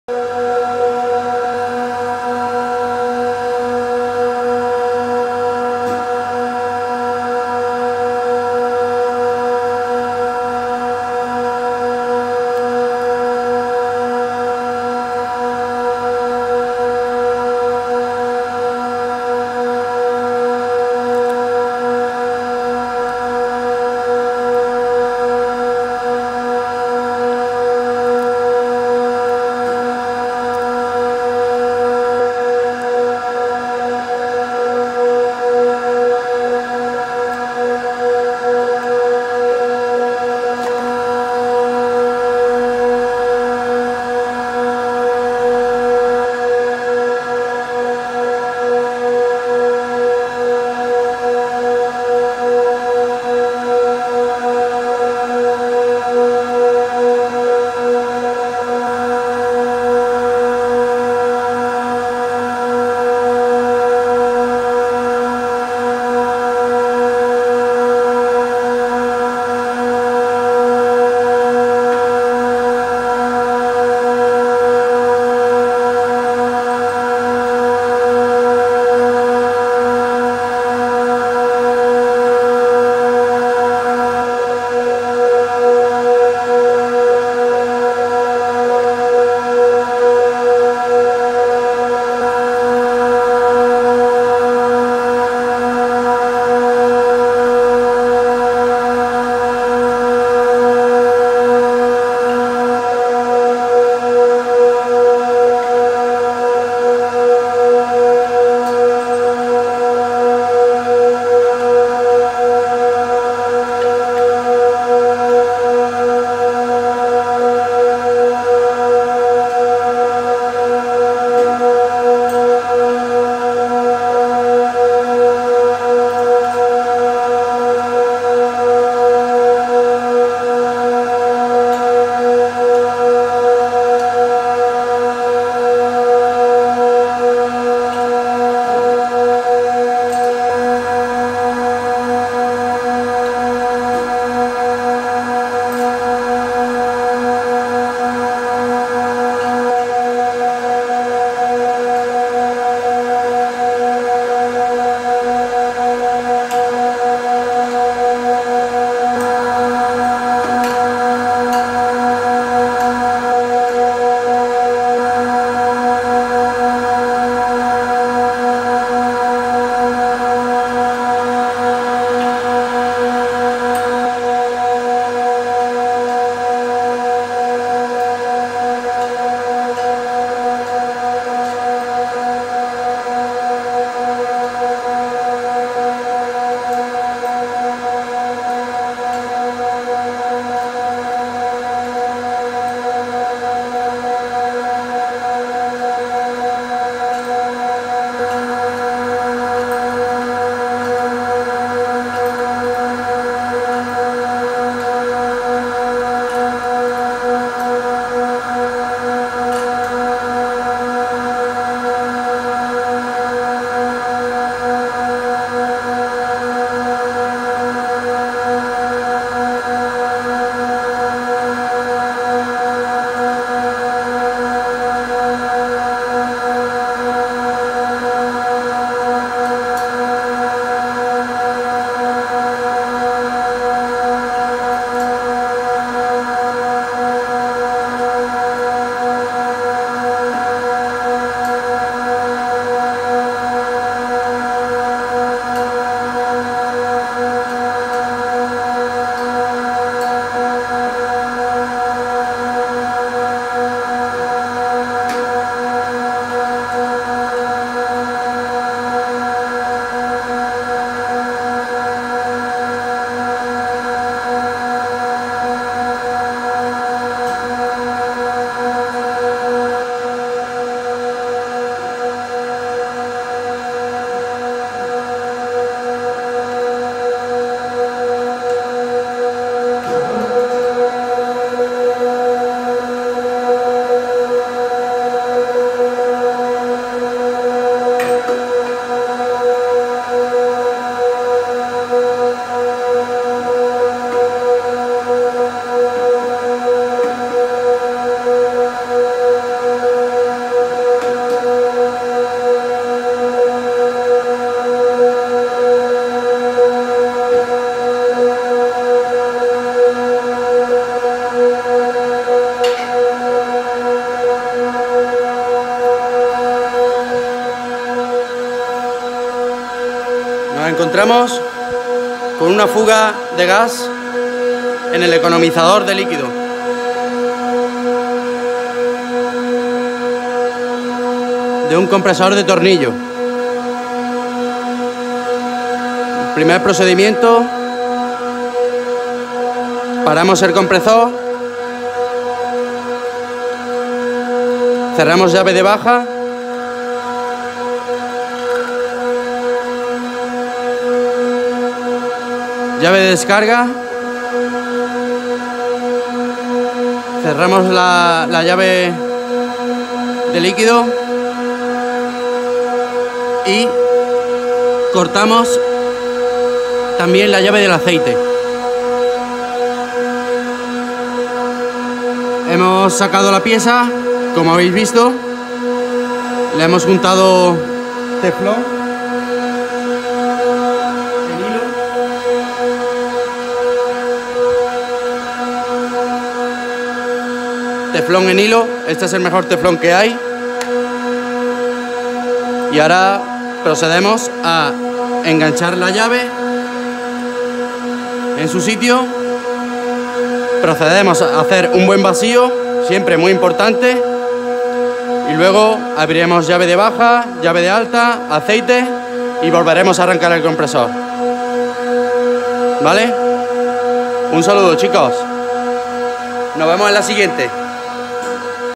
Averia compresor tornillo
Fuga de gas en tuerca rotolof de economizador